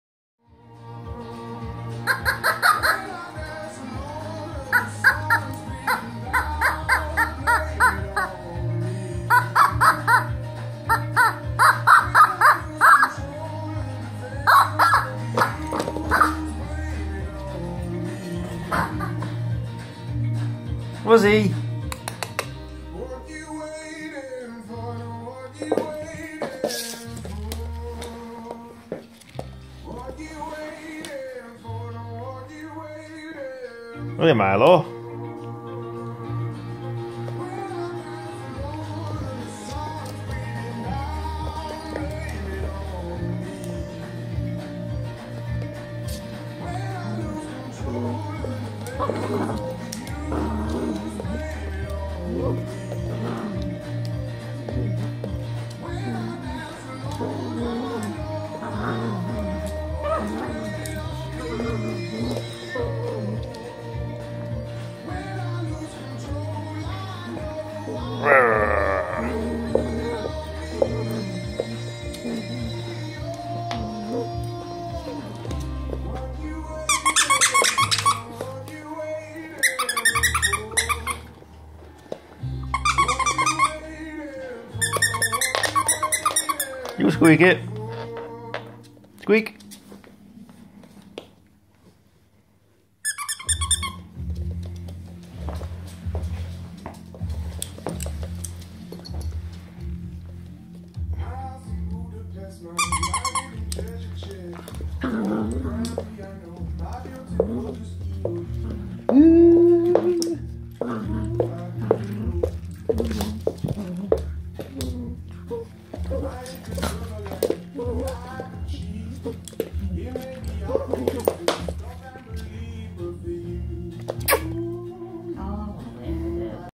Rosie the cavalier king charles playing
Rosie and me playing at 6 months